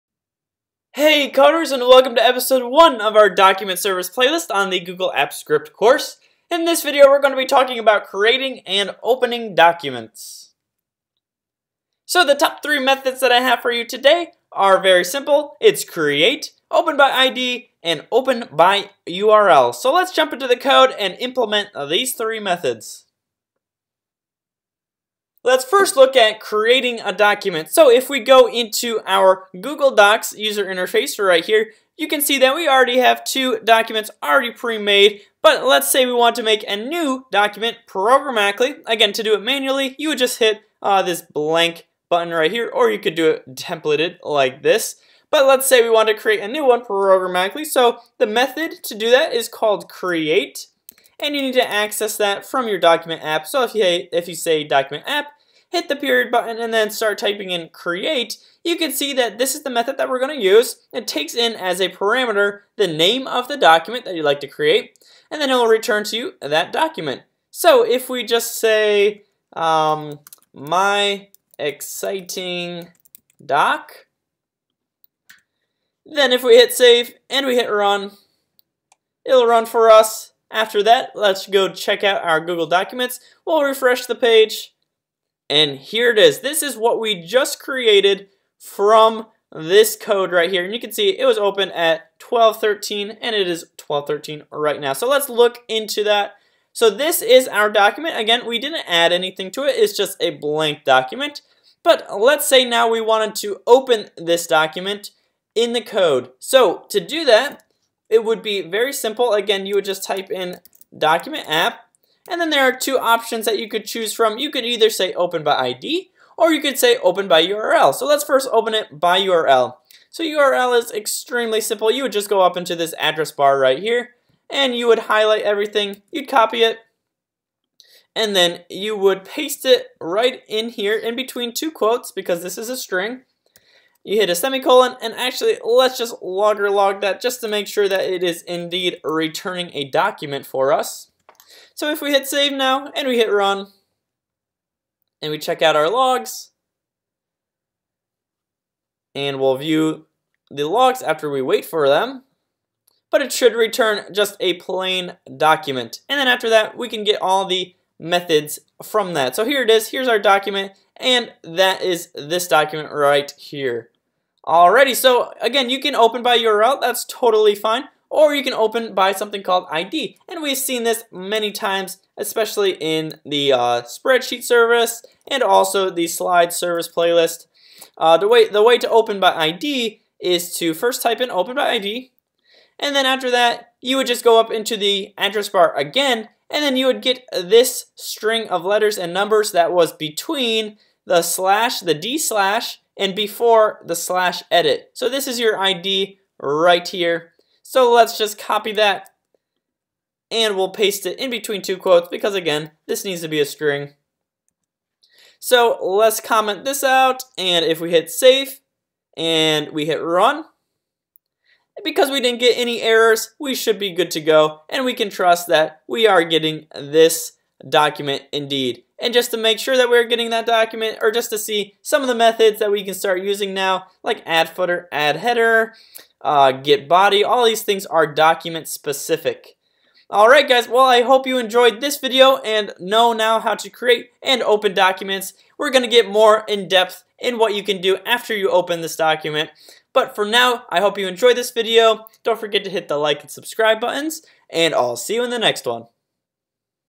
Create & Open Documents - Episode 6.1 | Apps Script ~ Document Service
Hey Coders! So you have authorized your app to use DocumentApp. Now what? The next step in your journey is to either open an existing Document, or create a new one from scratch! This video will show you how to do both so that you'll now have something to work with in your code.